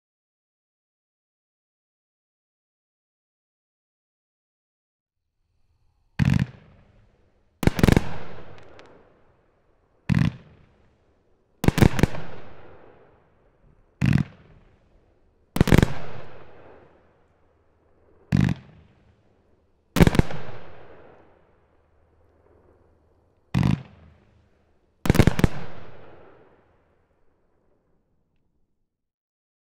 Rogue Nation 500g Fan Multi-shot Cake - Cutting Edge Fireworks
25 shots - Red flying stars, mine.  Peacock blue dahlia with red flying stars.  Green dahlia with red flying stars.  Silver coconut dahlia with red flying stars.